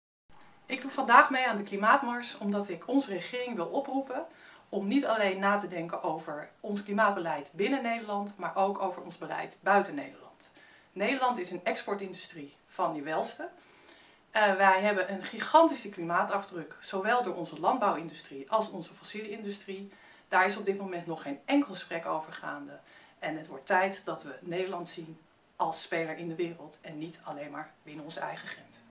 Both ENDS filmpje klimaatmars
Danielle Hirsch, directeur Both ENDS, vertelt waarom Both ENDS het belangrijk vindt dat er aandacht komt voor de rol van Nederland in klimaatverandering buiten onze grenzen.